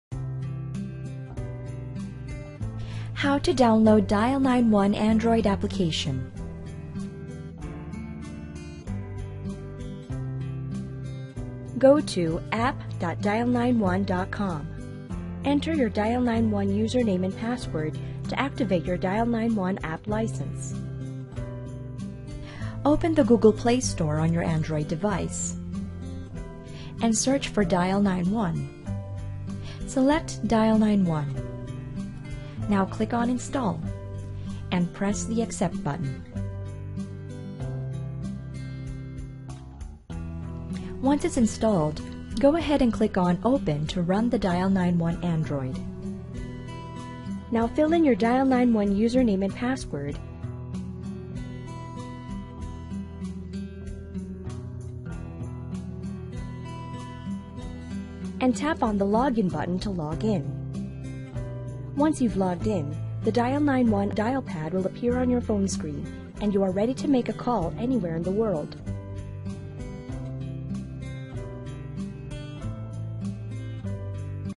How to install Dial91 Android App
Step by Step to install Dial91 Android App

1. Go to App.dial91.com, Enter your dial91 username and password to activate your dial91 Android app license

2. Then Go to Google Play and search for dial91

3. Tap on FREE then Tap on Install

4. Now app is installed, tap on OPEN to open the app.

6. Now fill your Dial91 Username and Password and tap on Login button to login

7. On press login button dial91 Android app dialpad appear on your phone screen and you are ready to make a call anywhere in the world